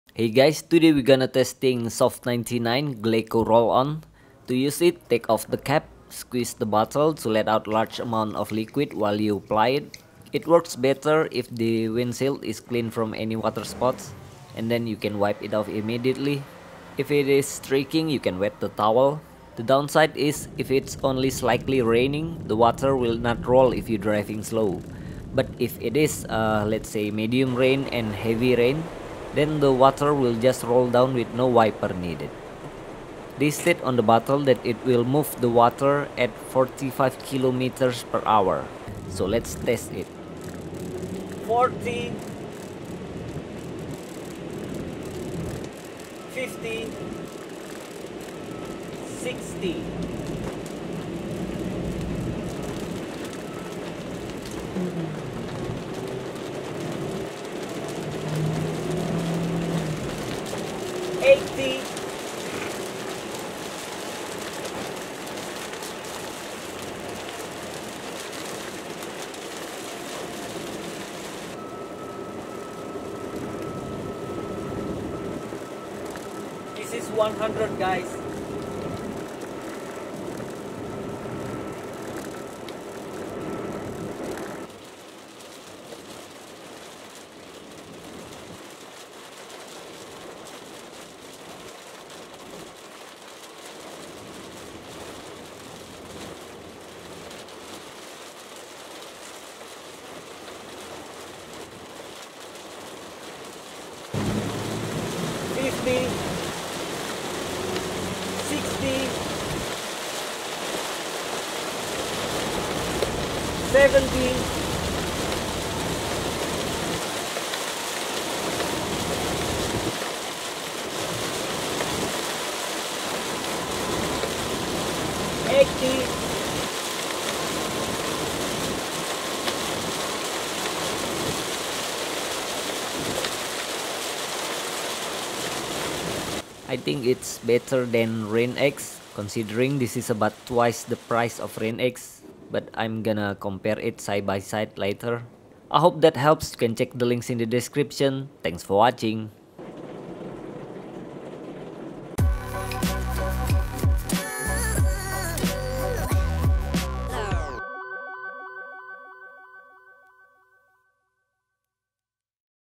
Windshield Rain Repellent : Soft99 Glaco Roll on
Tutorial, Review & Result!
Improves Visibility & Safety in Harsh Weather Conditions!

TOKO ONLINE SHOP ALFIE DETAILING